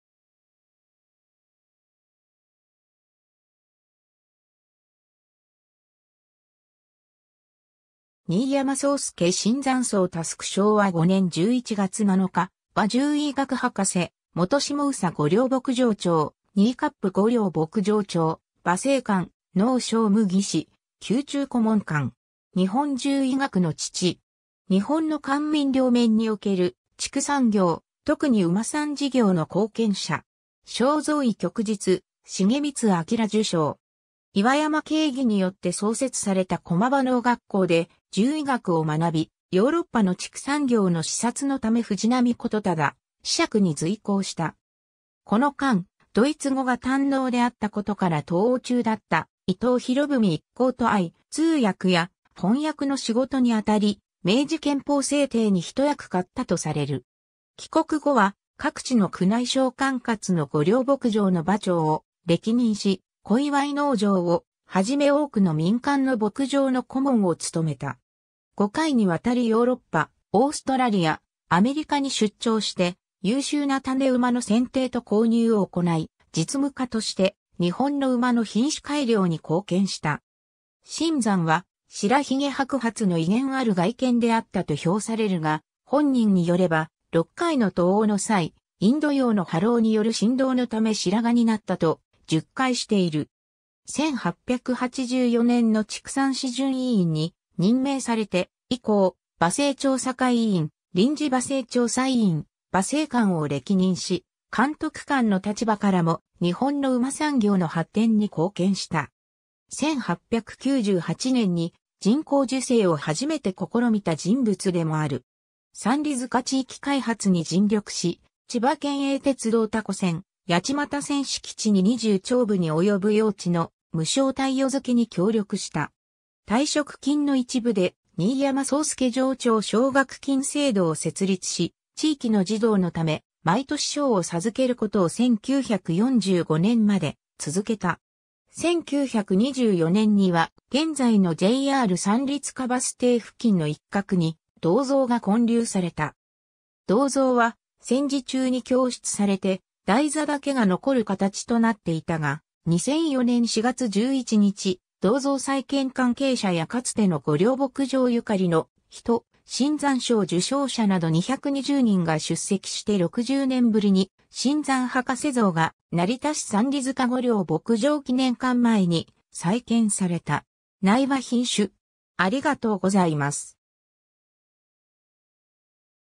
新山荘輔
新山荘輔（三里塚記念公園・成田市） 新山 荘輔（にいやま そうすけ、安政3年10月10日（1856年11月7日） - 昭和5年（1930年）11月7日）は獣医学博士、元下総御料牧場長、新冠御料牧場長、馬政官、農商務技師、宮中顧問官。
日本獣医学の父。
日本の官民両面における畜産業、特に馬産事業の貢献者。
正三位旭日重光章受章。
 岩山敬義によって創設された駒場農学校（後の東京大学農学部）で獣医学を学び、ヨーロッパの畜産業の視察のため藤波言忠子爵に随行した。
この間、ドイツ語が堪能であったことから渡欧中だった伊藤博文一行と会い、通訳や翻訳の仕事にあたり、明治憲法制定に一役買ったとされる。
 帰国後は各地の宮内省管轄の御料牧場の場長を歴任し、小岩井農場をはじめ多くの民間の牧場の顧問を務めた。
5回に渡りヨーロッパ、オーストラリア、アメリカに出張して優秀な種馬の選定と購入を行い、実務家として日本の馬の品種改良に貢献した。
 新山は白髭白髪の威厳ある外見であったと評されるが、本人によれば、6回の渡欧（当時は船）の際、インド洋の波浪による心労のため白髪になったと述懐している。
 1884年（明治17年）の畜産諮詢委員に任命されて以降、馬政調査会委員、臨時馬政調査委員、馬政官を歴任し、監督官の立場からも日本の馬産業の発展に貢献した。
 1898年（明治31年）に人工授精をはじめて試みた人物でもある。
 三里塚地域開発に尽力し、千葉県営鉄道多古線・八街線敷地に20町歩に及ぶ用地の無償貸与付けに協力した。
 退職金の一部で新山荘輔場長奨学金制度を設立し、地域の児童のため、毎年賞を授けることを1945年（昭和20年）まで続けた。
 1924年（大正13年）には、現在のJR三里塚バス停付近の一角に銅像が建立された。
銅像は戦時中に供出されて台座だけが残る形となっていたが、2004年（平成16年）4月11日、銅像再建関係者やかつての御料牧場ゆかりの人、新山賞受賞者など220人が出席して60年ぶりに新山博士像が成田市三里塚御料牧場記念館前に再建された。
 （）内は品種